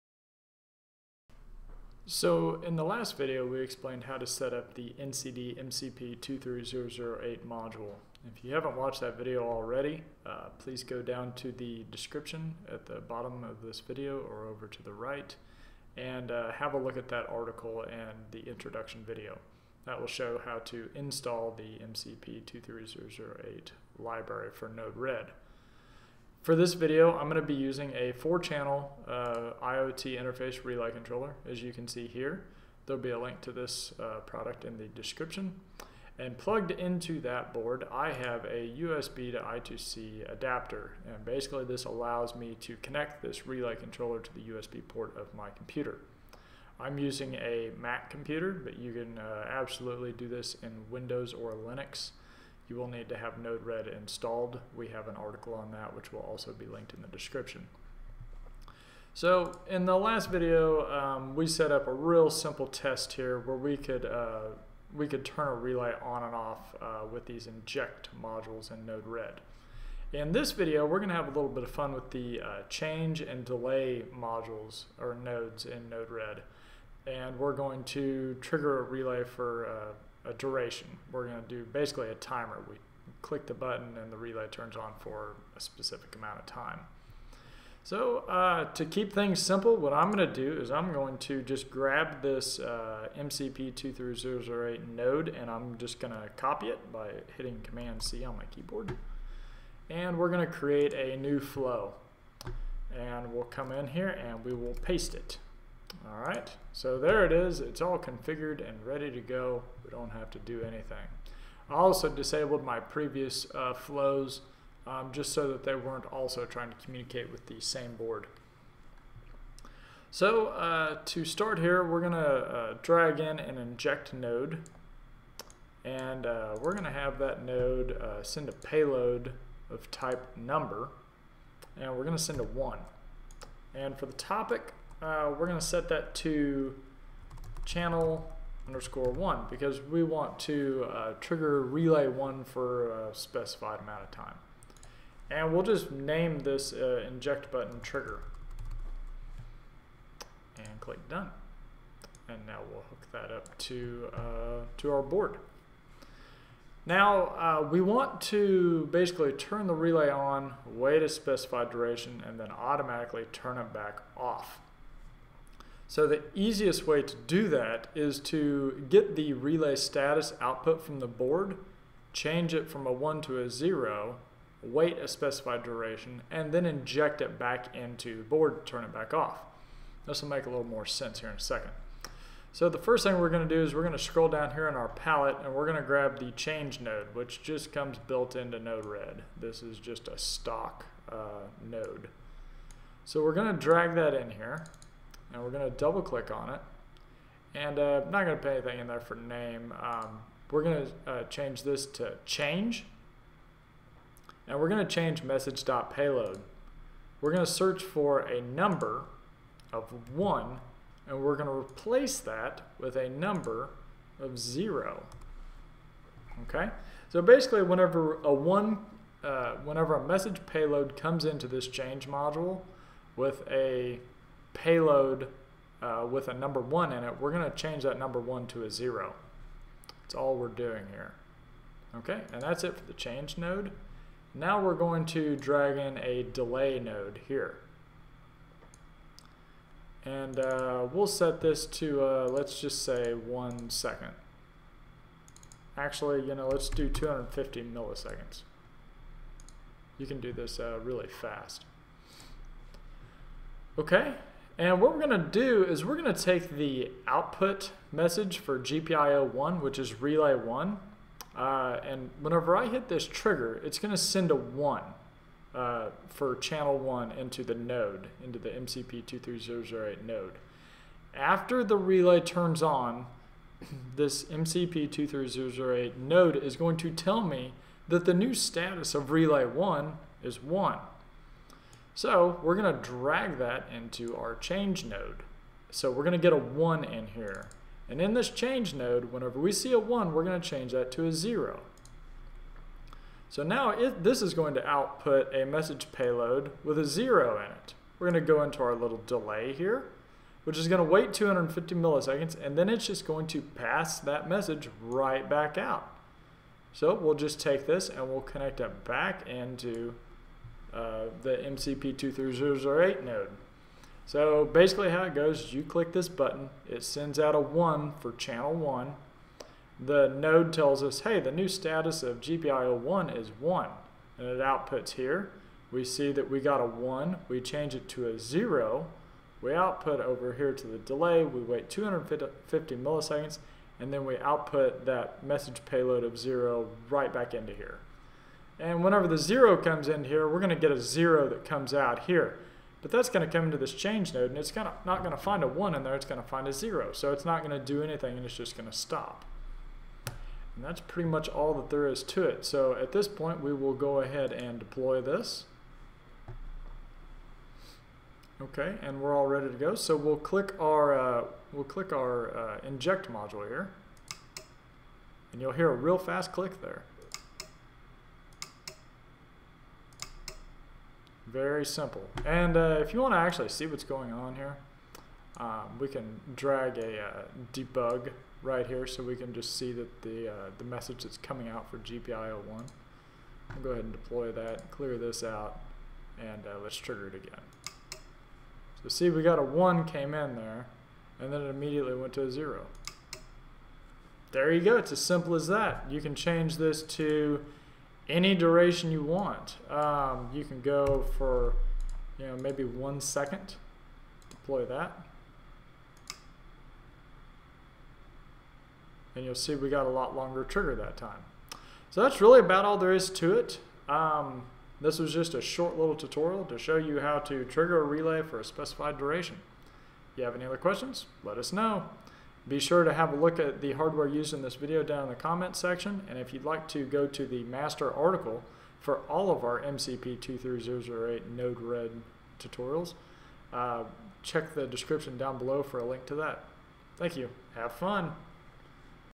Node Red MCP23008 Timer Tutorial
In this video, we will use NCD's MCP23008 Node Red module. We will trigger a relay for a specified duration using out-of-the-box functionality in Node Red. Check out our master article for Node-Red and the MCP23008 along with many other tutorials